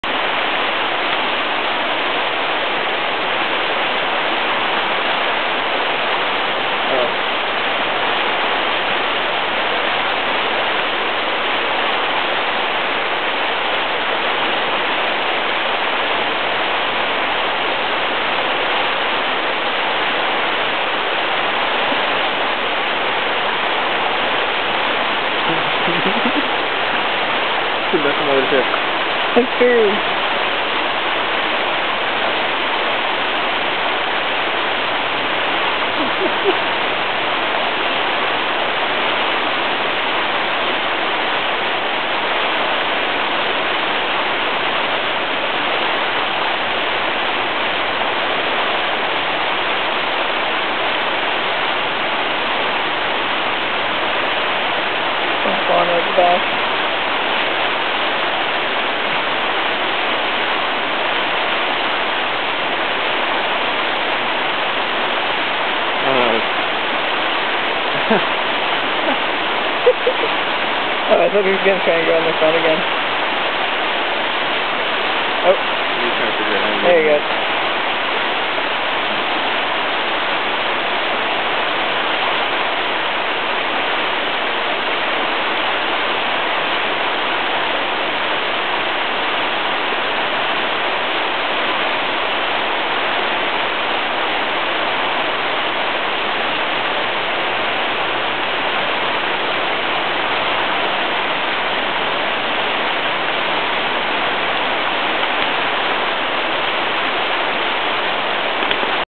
Duckies
Baby ducks, ready for a nap, cuddle under mom.